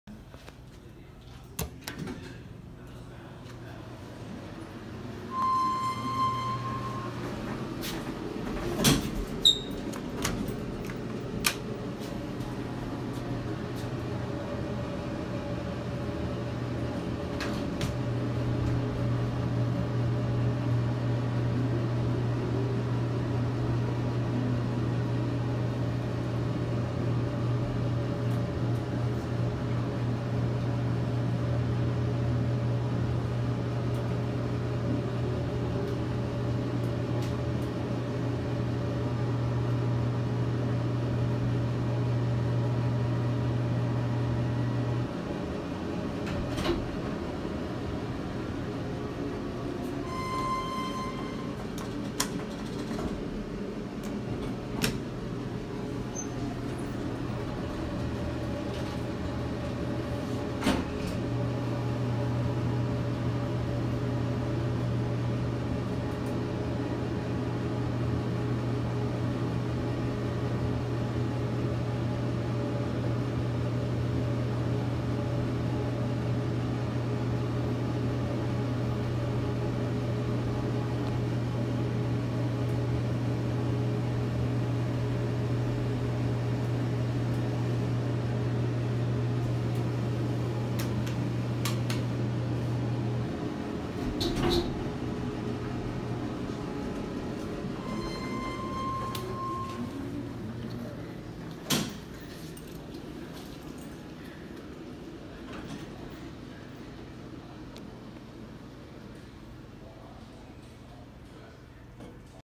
Mowrey Hydraulic Elevators at Hyatt Place
Riding the elevators at the Hyatt Place in Greenville, SC. The other elevator was broken so we couldn’t ride that one.